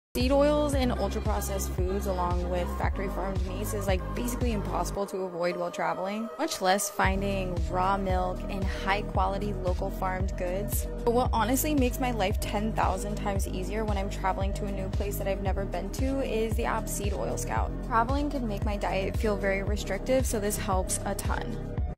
Seed Oil Scout is a great resource to find clean food near you!
So grateful for @seedoilscout helps me anywhere in the world to find places that I can get quality, ethical foods. Even here in Mexico 🤭

@swrfarmersmarket is a godsend and makes me contemplate moving back to the other coast.